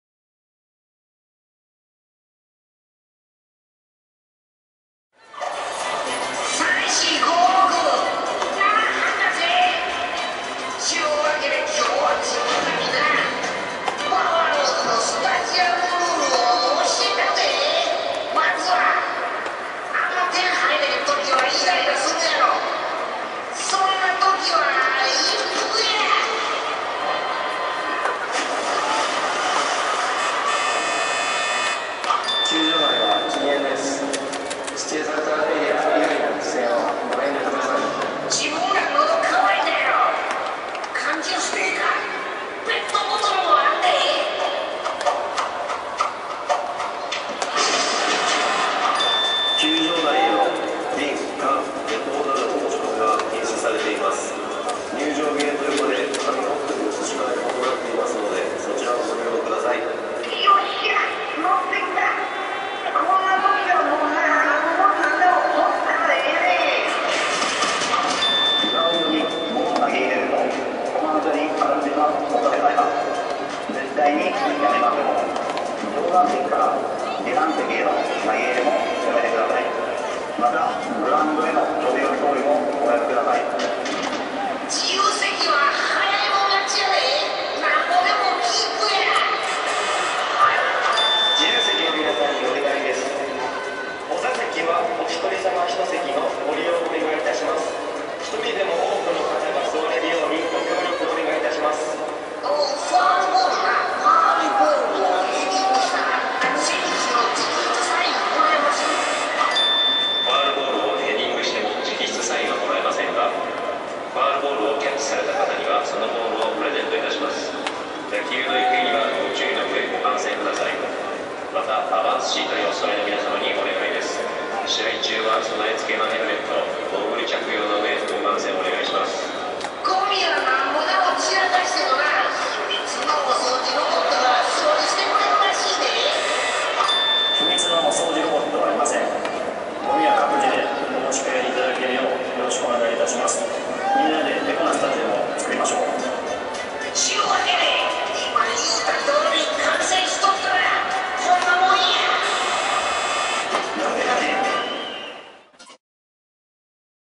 2011 Bs スタジアムルール＠京セラドーム 八カセ ブル ベル
2011年の京セラドームでのオリックス・バファローズ主催試合にビジョンで流れるスタジアムルール。八カセとバファローブル＆バファローベルのコンピュータグラフィック映像。